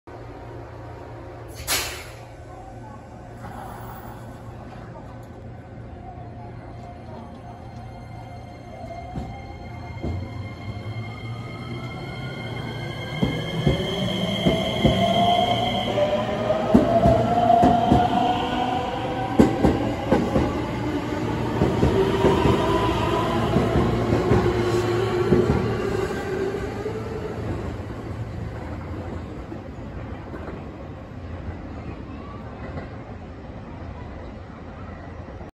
『朝と夕方に運行されている丹波路快速を撮影』G丹波路快速大阪行き 225系6000番台 ML04編成 6両編成
朝と夕方に運行されている
丹波路快速大阪行きを撮影しました。
運良く6両編成の
225系6000番台を撮影しました。

2022年内に撮影

行き先・種別　　丹波路快速大阪行き

車両・編成

225系6000番台 ML04編成

また最後になりますが実は前から
Twitterをやめますと
言ってましたが申し訳ございません。
別のTwitterアカウント作りました。